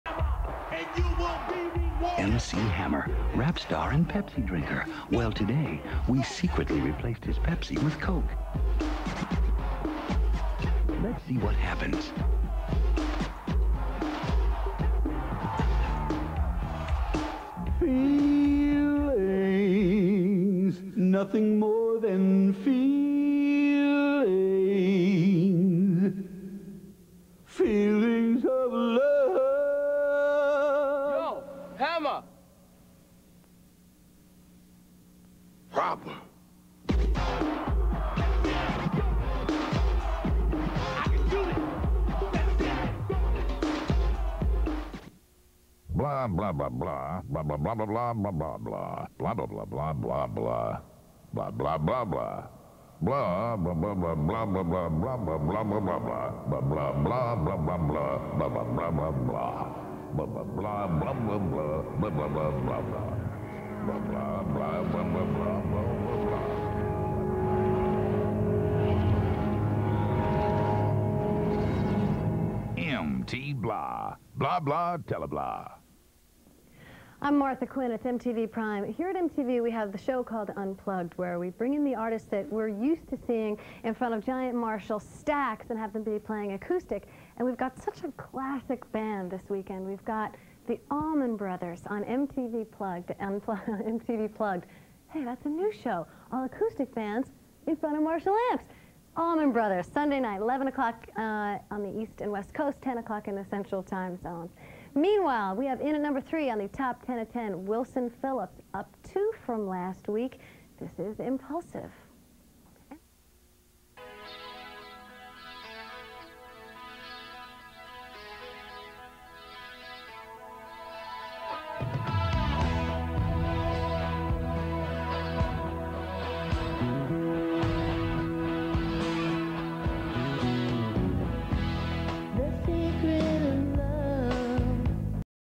MTV Martha Quinn Classic ads
Mtv Prime was the near end of the great Mtv days.  Great MC Hammer Pepsi spot with MTV Blah Blah Teleblah ad... good stuff.